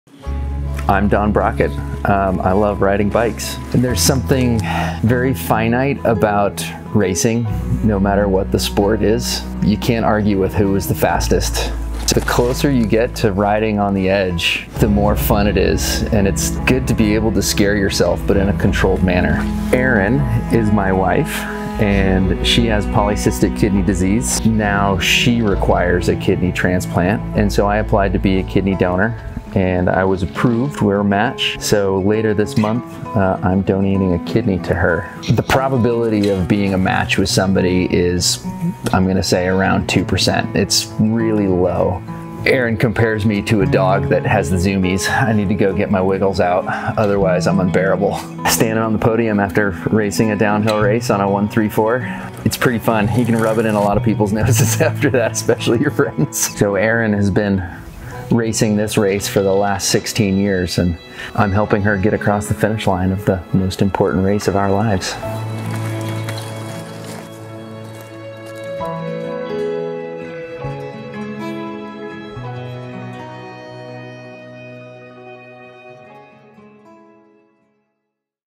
My Kona - Don Brockett
Don Brockett is a racer. He used to race in alpine ski events at an elite level. He raced street bikes on track. Now, he races downhill courses, but manages to pull off the W aboard his trusty Process 134. Now Don's in a much more critical race. His wife, diagnosed with Polycystic Kidney Disease and requires a kidney transplant. With the likelihood of Don being a match at less than 2%, the future was uncertain. But like every other part of their story, the universe lined up in a magical way and Don is a match. On July 28th, he gave her a kidney. Here is their story.